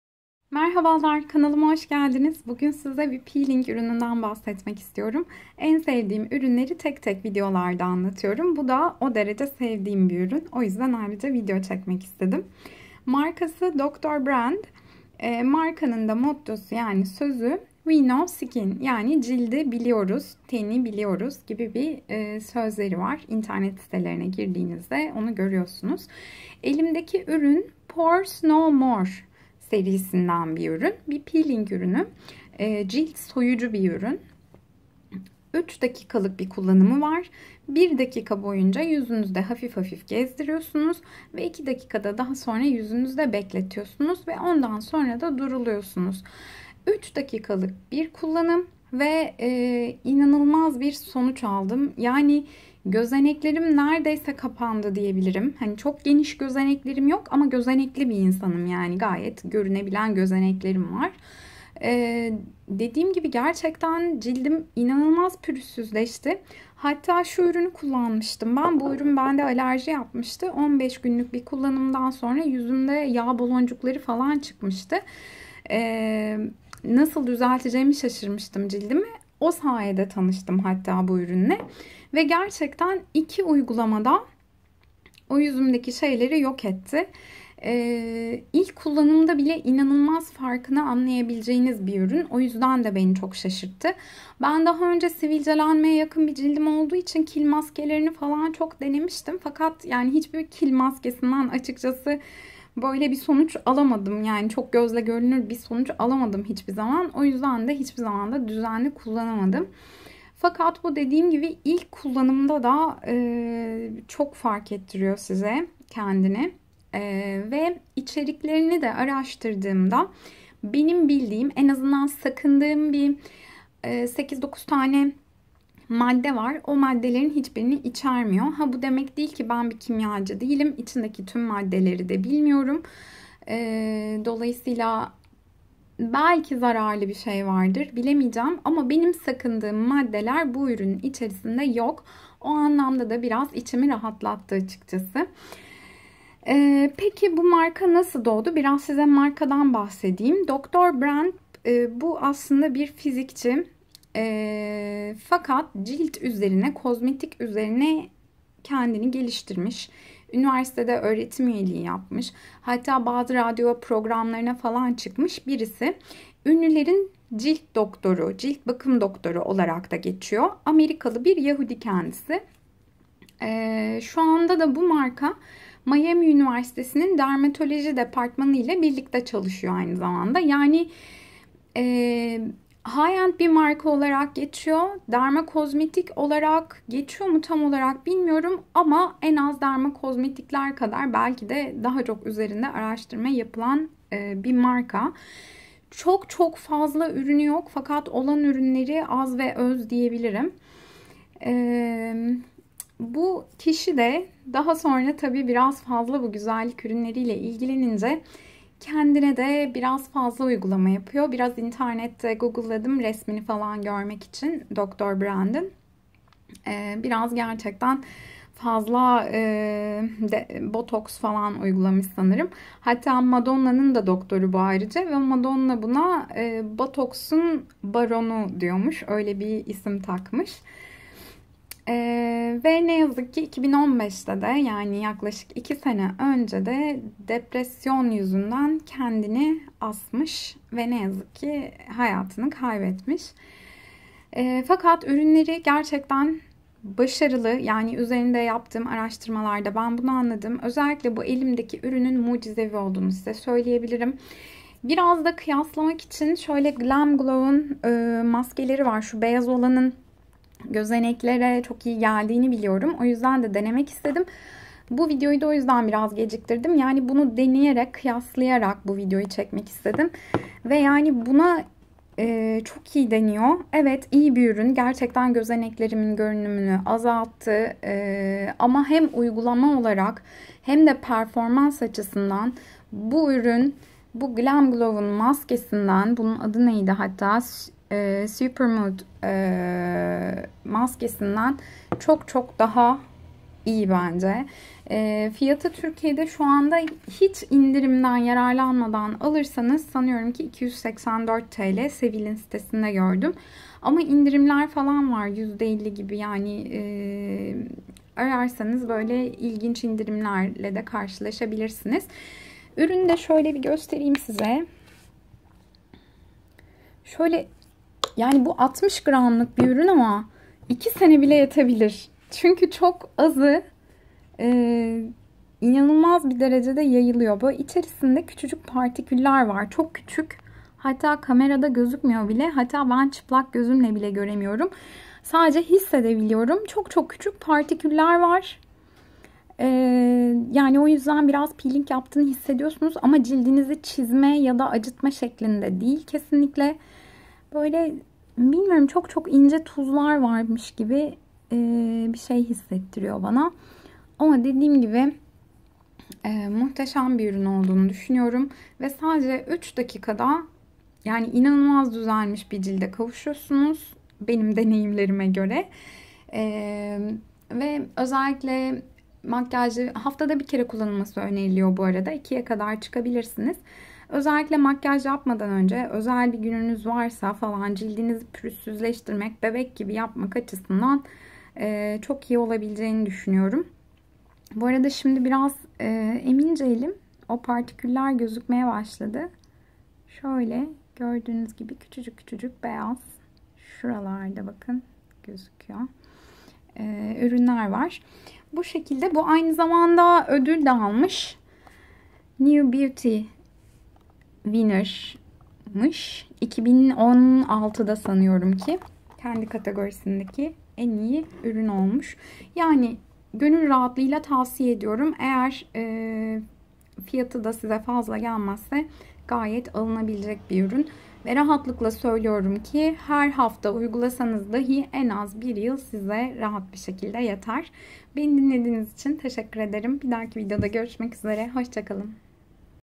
PoreDermabrasion || dr. brandt || peeling urunu
Glamglow supermud ile karsilastirma.
PoreDermabrasion peeling urunu bu gune kadar kullandigim en iyi gozenek gorunumunu azaltici urun.